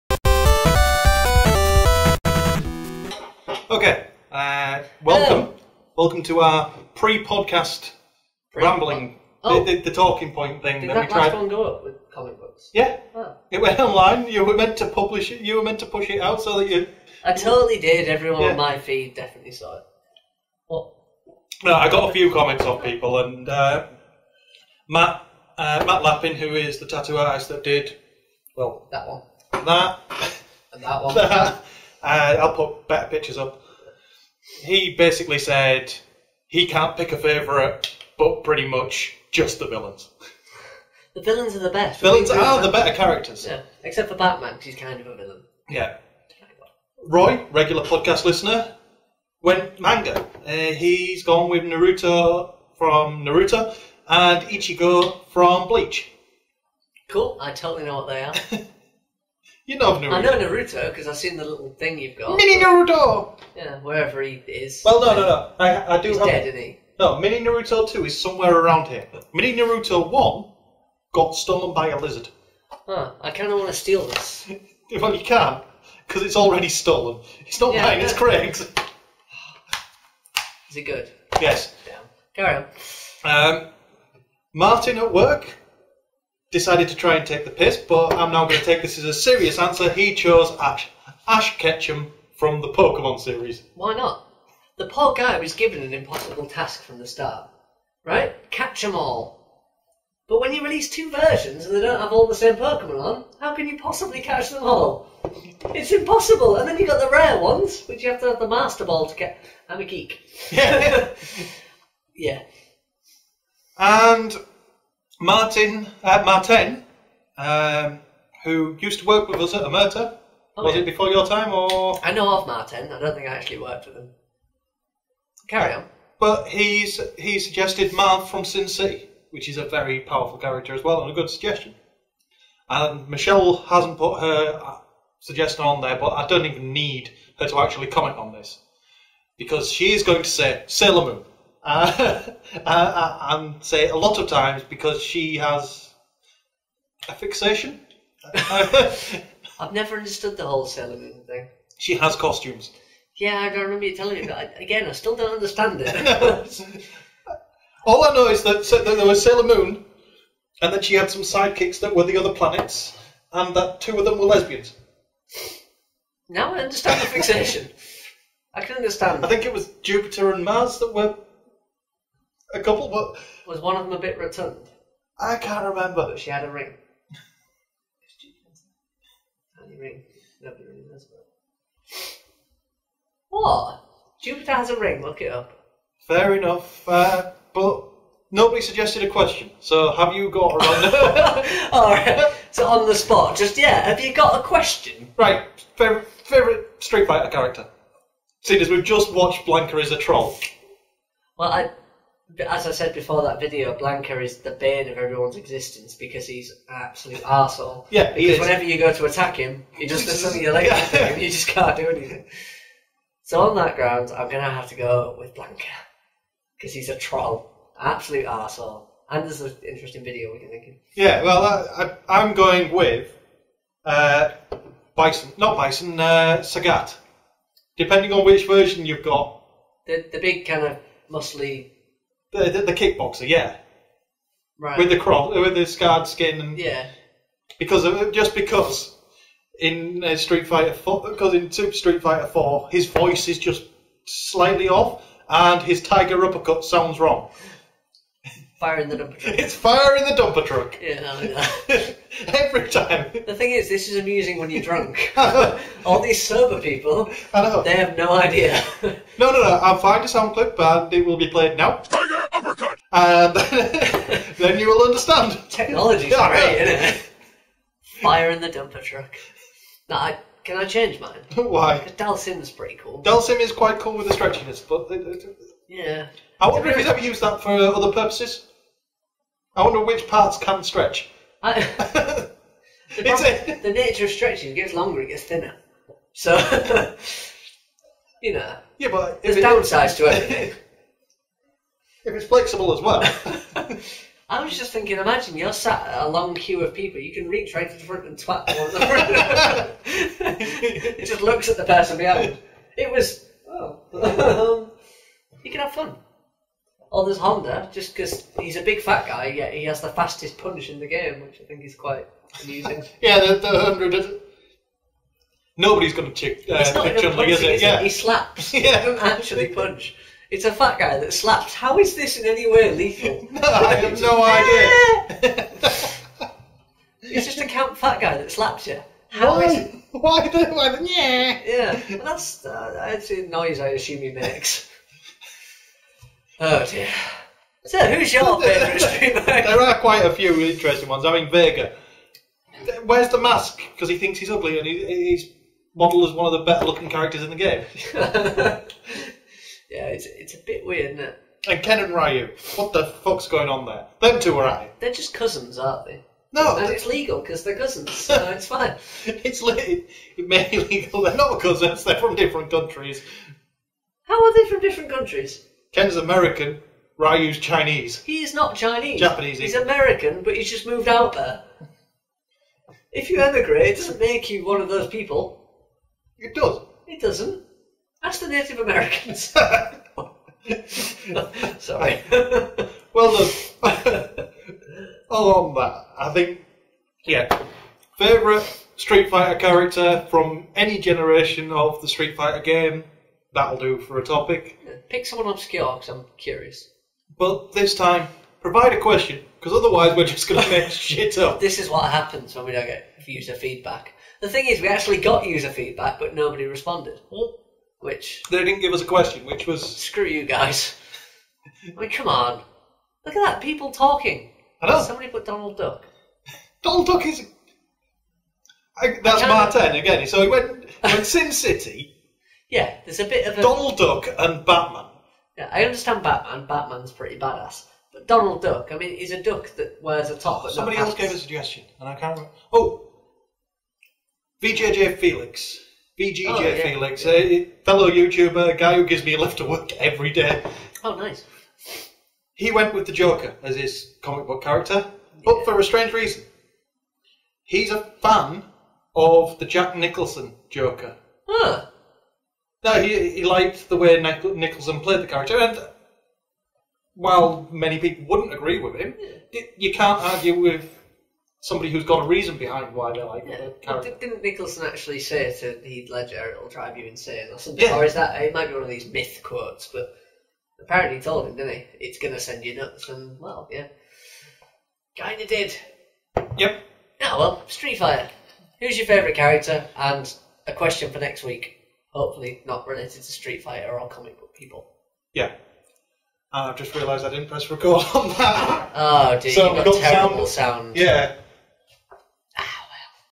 Talking point: Which is your favourite Street Fighter Character?
For the second week as a warmup for the Drinky Talky Thingy Podcast, we recorded a Talking Point video.

FIrst we caught up on the suggestions made by the Listeners & Viewers of the previous show, here were their favourite Comic Characters.

- Matt Lapping (Creative Vandals - Couldn't pick an individual but shared the opinion that the villains are the better characters.
- Shelly Woo Chatterton - Sailor Moon and she said it a lot of times as she may be a bit of a fan
- VGJFelix - Joker, because he liked the way that Jack Nicholson played him
- Roy Bass - Naruto from Naruto or Ichigo from Bleach
- Martin Tattersall - Ash Ketchum from the Pokemon
- Maarten Solinger - Marv from Sin City, badass with twisted standards that he alone lives by, but consistently. Ow, and Donald Duck and Batman

This week as we didn't have a questions selected for us, we went with another simple question - Who is your favourite StreetFighter Character. This was because we had just watched the following video.

Everything is fair game, from politics to movies, music to games, hell we will even discuss our favourite womble if you so desire it.

Simply let us know below.